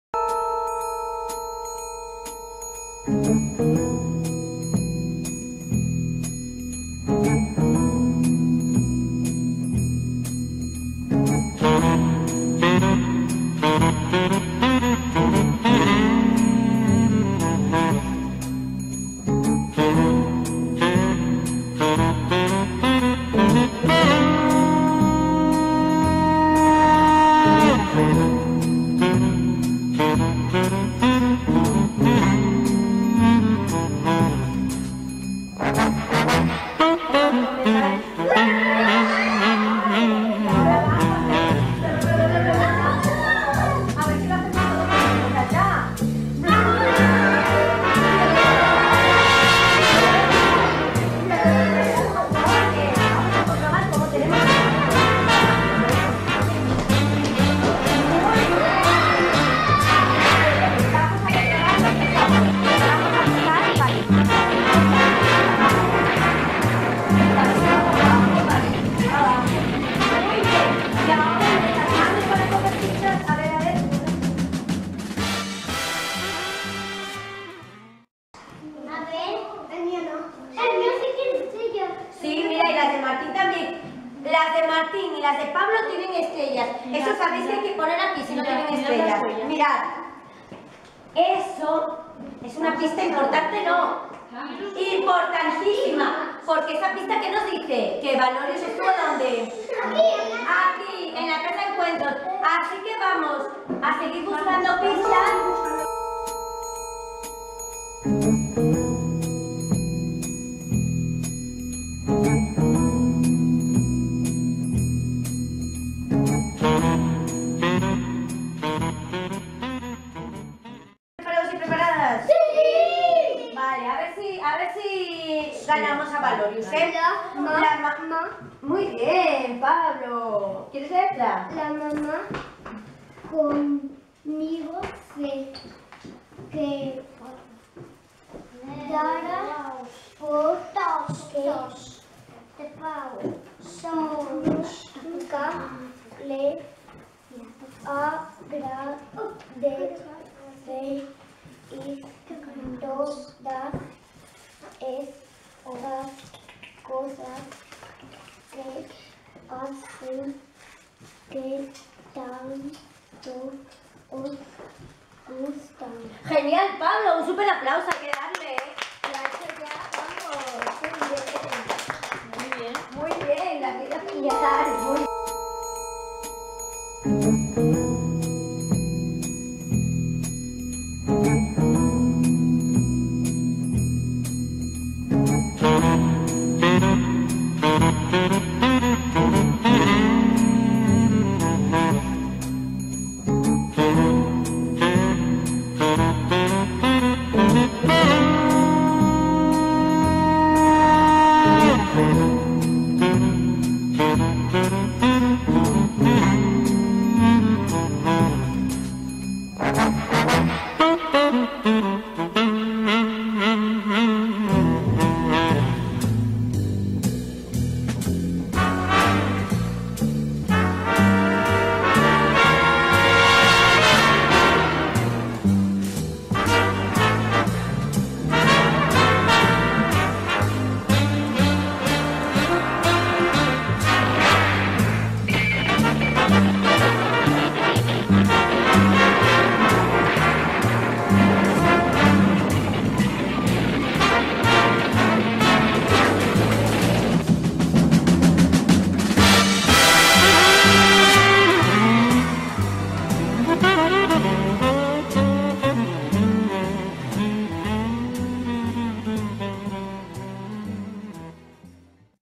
BUSCANDO A VALORIUS
Las niñas y niños de 5A visitamos la casa de encuentros de las mujeres de Castrillón.